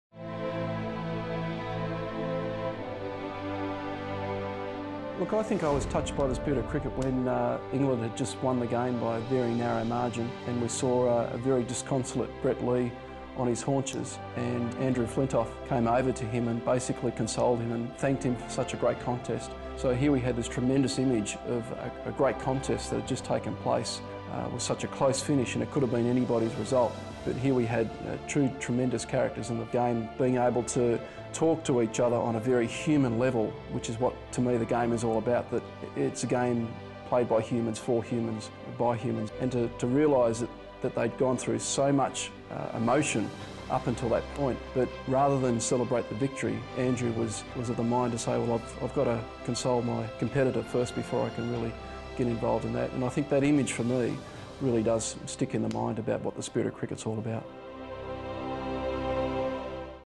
Simon Taufel - The Spirit of Cricket
Simon Taufel talks about the spirit of Cricket